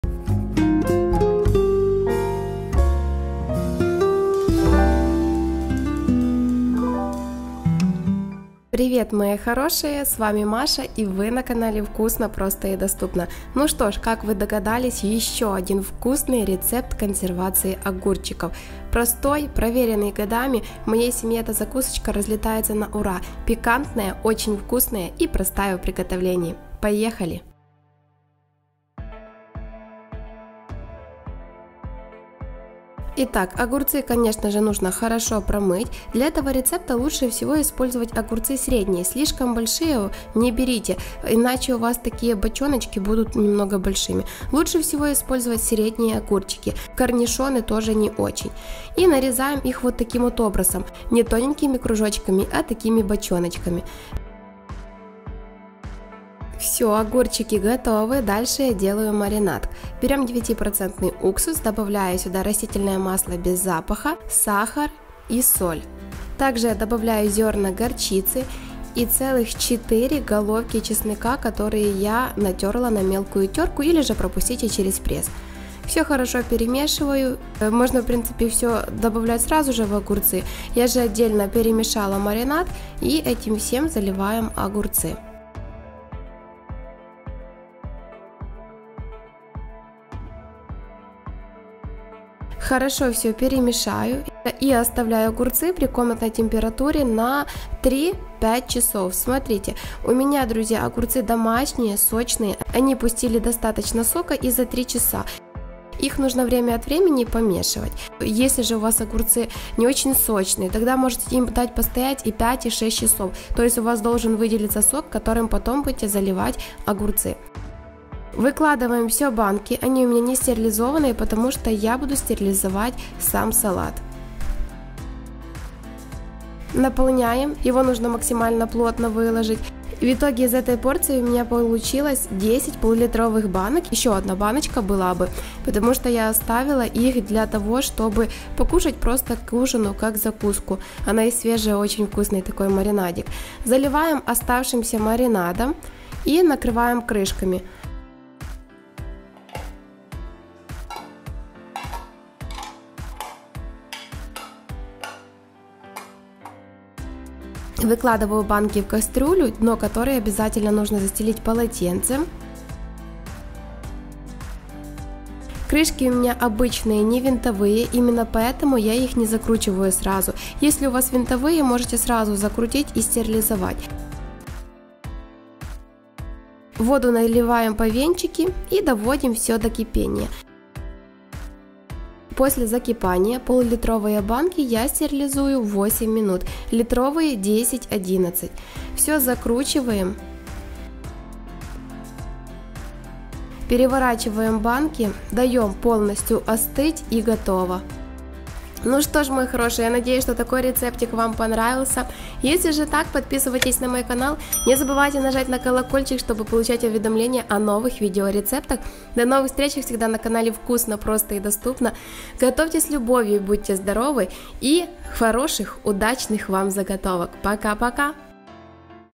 КАЖДЫЙ ГОД ГОТОВЛЮ ДВОЙНУЮ ПОРЦИЮ! ВСЯ СЕМЬЯ В ВОСТОРГЕ ОТ ЭТИХ ХРУСТЯЩИХ ОГУРЧИКОВ на Зиму!
Всем привет! Сегодня на канале "Вкусно, просто и доступно" я покажу ещё один способ, как замариновать огурцы на зиму. Этот способ очень простой и проверен годами! Я подробно покажу весь процесс, а также поделюсь с вами рецептом интересного маринада с горчицей, с ним огурцы получаются хрустящими и очень вкусными.

Нам нужно:
-огурцы - 4 кг.
-горчица в зернах - 20 гр
-чеснок - 4 головки (150 гр)
-перец чорный молотый - 2 ч.л. (5 гр.) я не добавляла

На сайте (Доставка по всему миру)